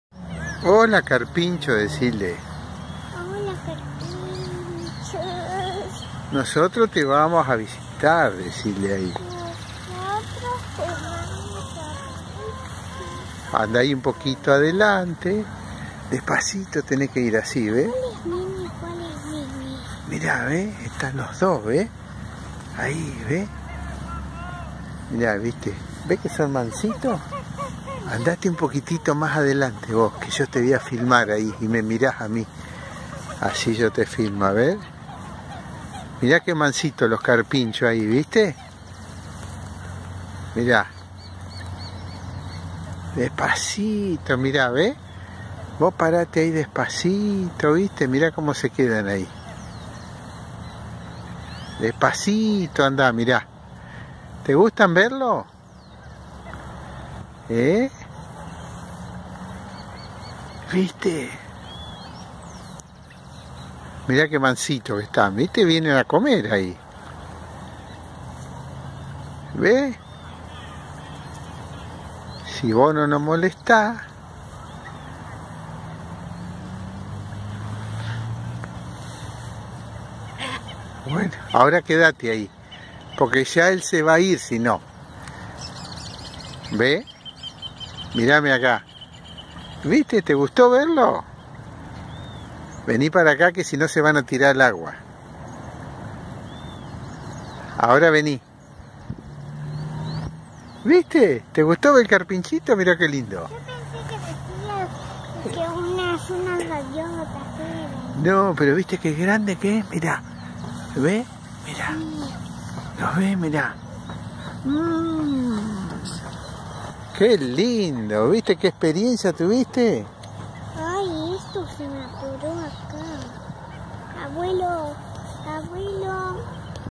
Naturaleza y Vida Silvestre
Experiencia de apreciar carpinchos en su hábitat - Laguna del Difunto Manuel - Las Flores Pcia. Bs. As - a